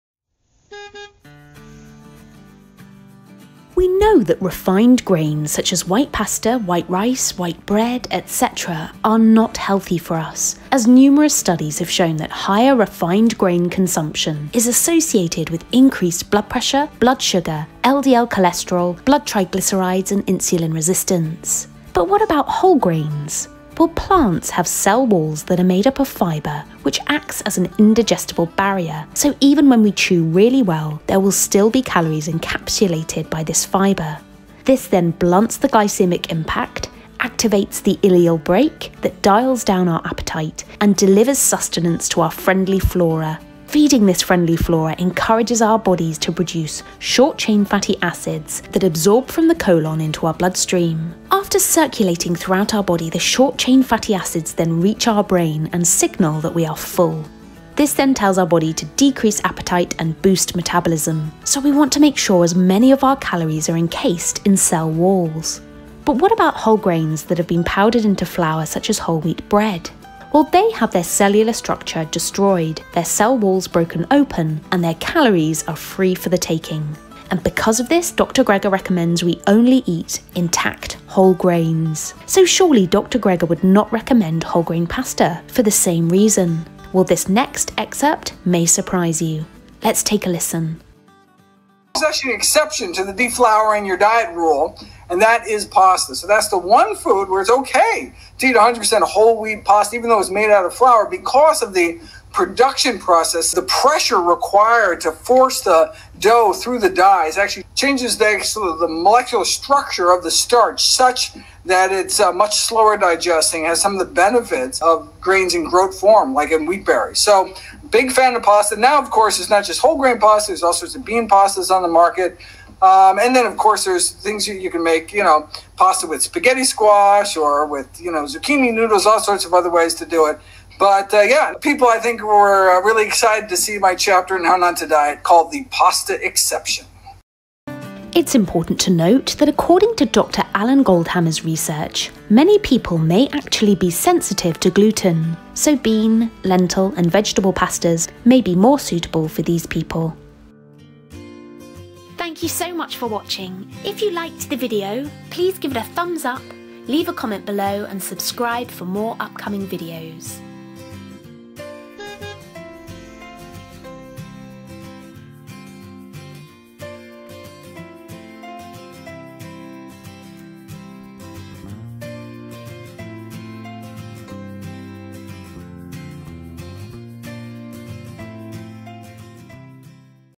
Is Pasta Good Or Bad For Us? What Does The Science Say? Dr Greger
We know that refined grains are not good for us but about whole grains and in particular whole wheat pasta? Dr Michael Greger says we should try to de-flour our diets which means taking flour products out such as whole wheat bread. So what does the science and therefore Dr Greger say about pasta? Is it healthy for us or should we cut pasta out of our diets? Let's find out!

Here are the studies

Refined grain consumption and the metabolic syndrome in urban Asian Indians (Chennai Urban Rural Epidemiology Study 57)

Rice and noodle consumption is associated with insulin resistance and hyperglycaemia in an Asian population

Dietary carbohydrates, refined grains, glycemic load, and risk of coronary heart disease in Chinese adults